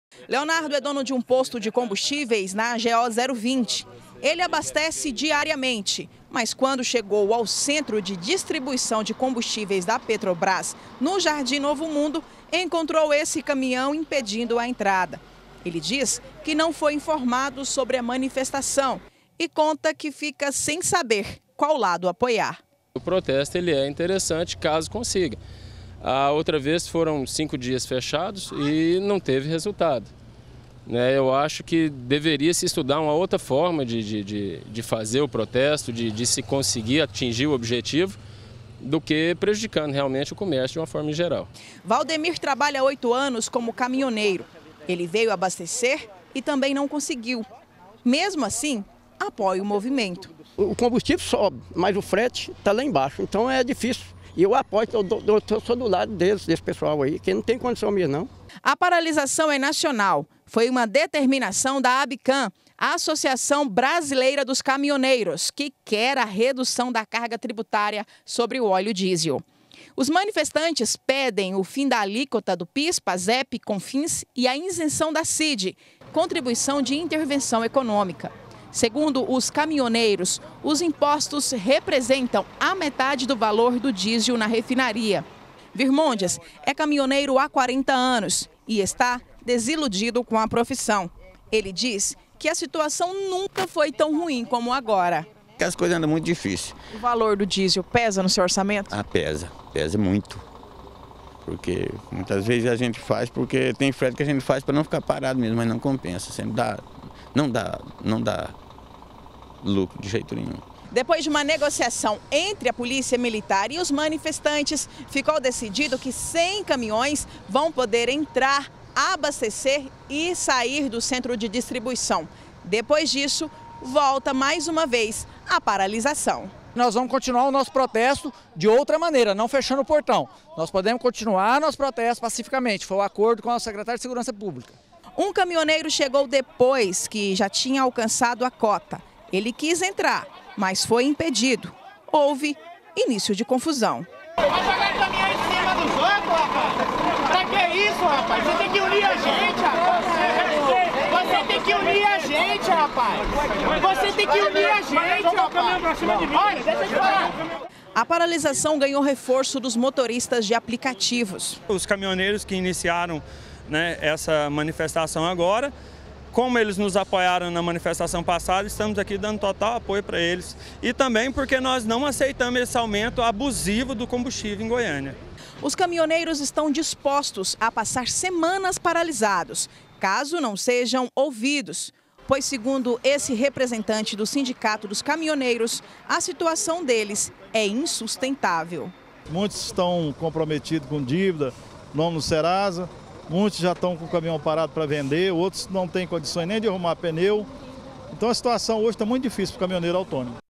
CA - Paralisação nacional dos caminhoneiros: Protesto contra impostos do óleo diesel - 21-05-2018
Indignados com mais um anúncio de aumento no preço do diesel, caminhoneiros bloqueiam distribuidora de combustíveis e várias rodovias pelo estado.